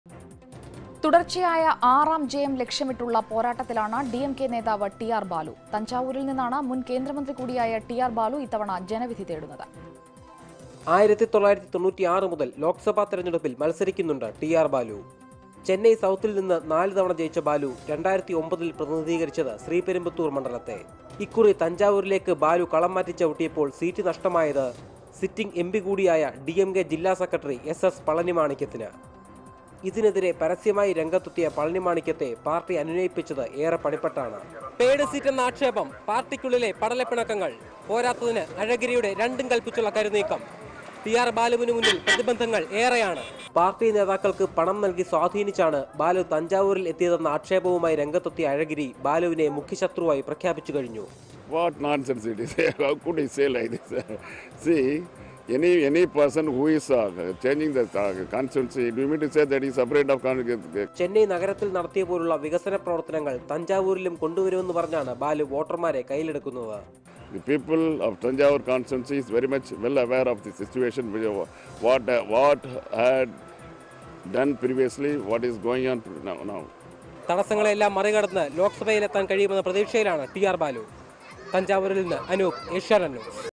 India Election 2014: T R Balu election campaign in Tanjavur
TR Baalu express his winning hopes